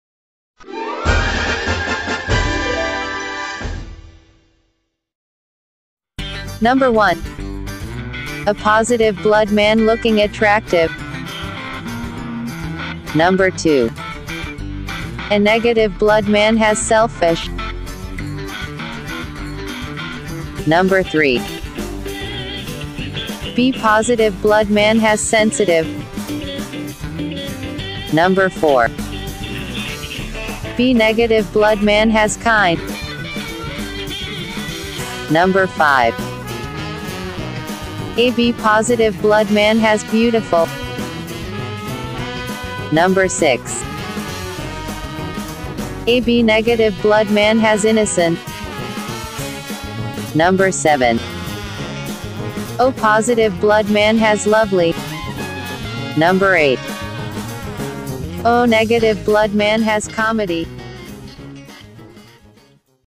Top Entertaining video | Man Behaviour According to Blood Type | Top Volco
In This Video we explained man behavior according to Their Blood Type A+ A- B+ B- .